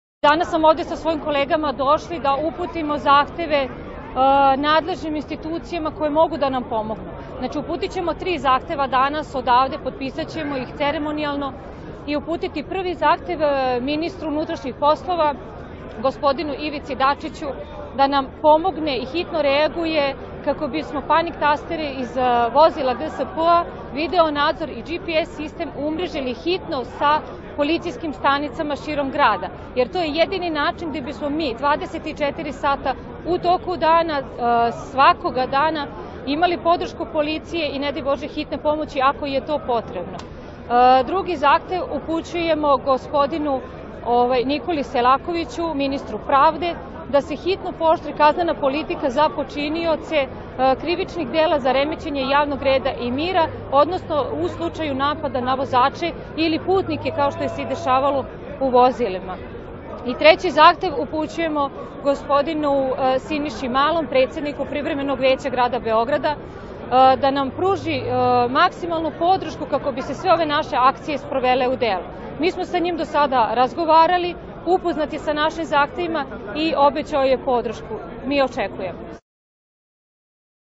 PROTEST SINDIKATA - SLOGA GSP - MIRA BASTA
Mila Basta, predsednik  Sindikata  Sloga- GSP,  najvećeg  reprezentativnog sindikata GSP, rekla je da je cilj  protesta da se skrene pažnja na njihove probleme i da se upute zahtevi nadležnim institucijama koje mogu da pomognu.
Uputićemo tri zahteva, kazala je ona, od kojih jedno ministru policije da se -  panik  tasteri umreže sa svim policijskim stanicama u gradu -- kako bi se odmah i hitno reagovalo na poziv, drugi  ministru pravosuđa da se pooštri kaznena politika  i treći zahtev Privremenom veću Beograda da nas podrži u našim zahtevima.
Neprimereno je da čovek koji želi da zaradi za parče hleba dobije batine od huligana  kojima je to hir, kazala je Basta, ali i najavila mogućnost radikalizacije ukoliko nadležni ne budu adekvatno  odreagovali  na njihove zahteve.